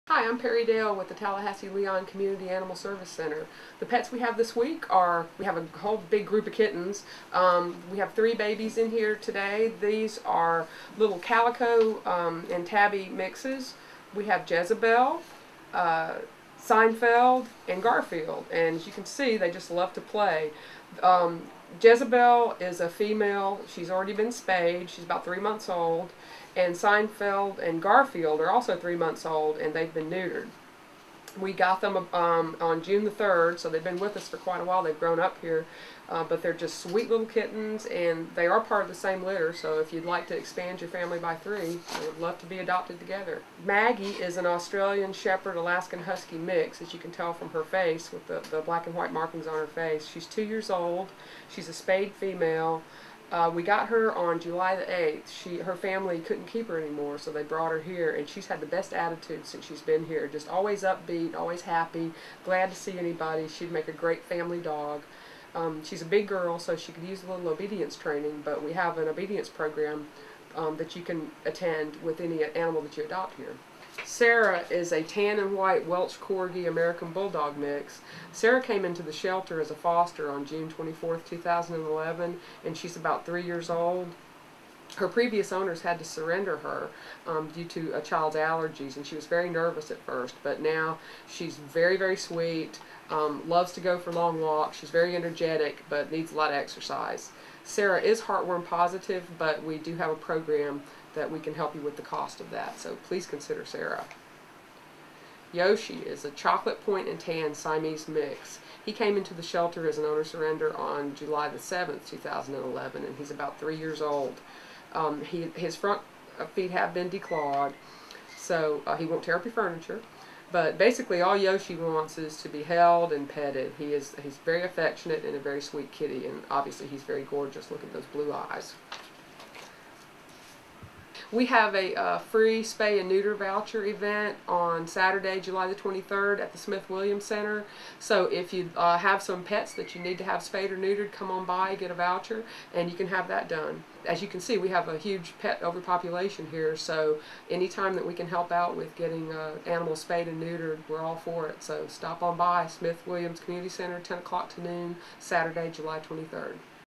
7/13: Rescue Me Adoptable Pets
Meet this week's adoptable pets awaiting their chance to find a loving home! Also hear about the upcoming free spay & neuter voucher event going on Saturday, July 23 from 10am-noon at the Smith-Williams Center.